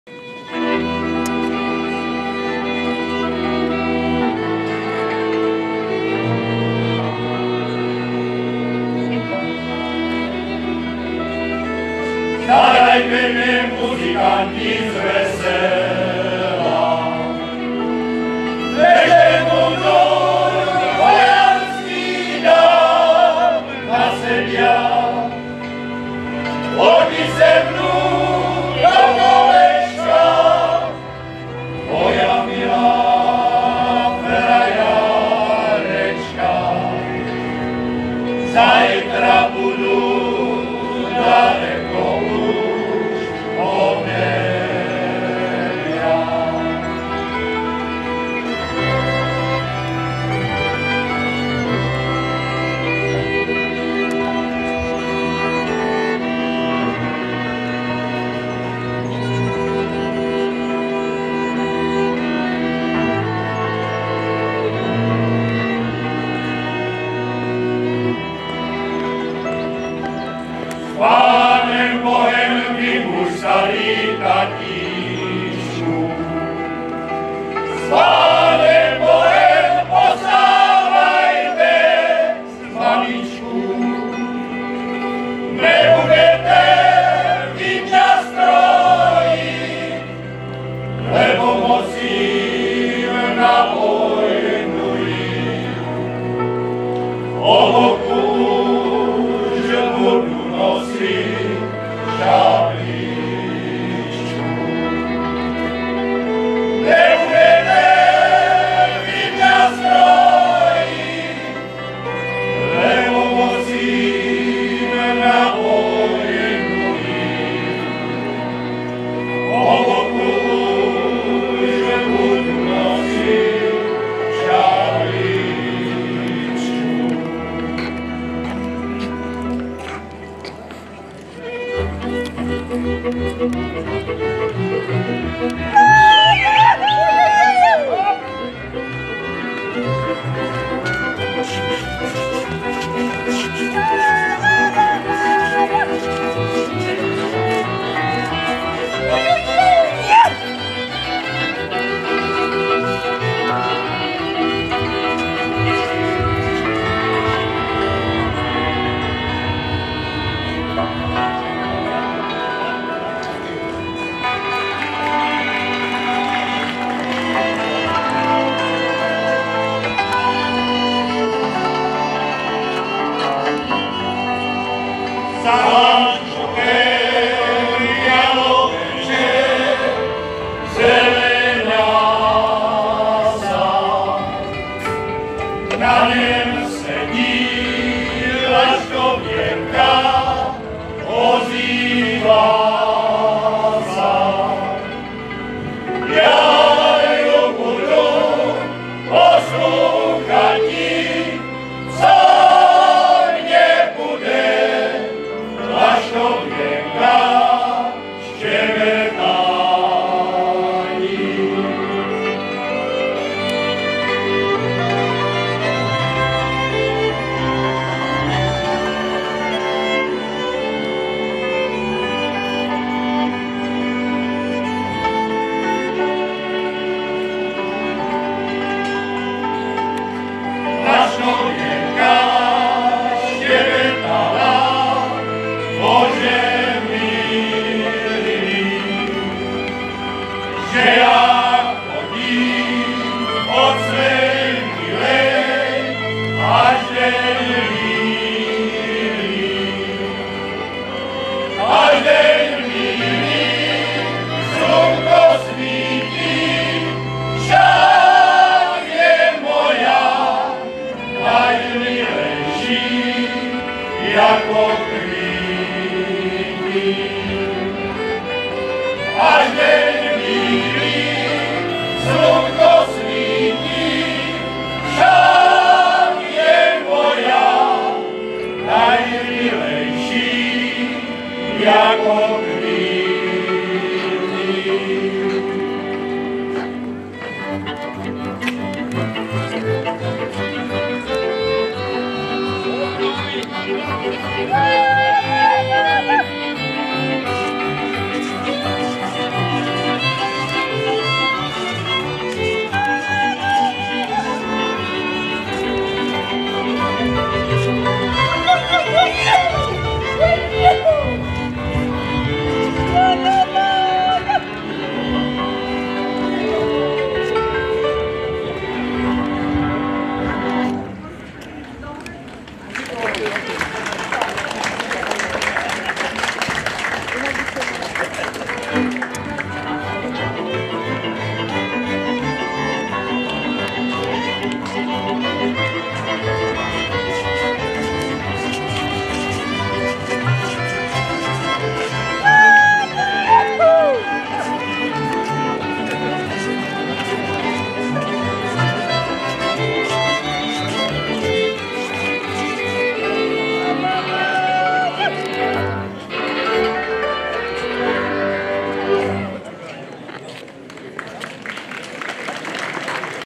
DOLNÍ DUNAJOVICE-13.SETKÁNÍ MUŽSKÝCH SBORŮ (Mužácký sbor z Dolních Dunajovic potřetí)
- 3. července 2021, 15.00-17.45 hodin, přírodní areál uprostřed obce Dolní Dunajovice, Mikulovsko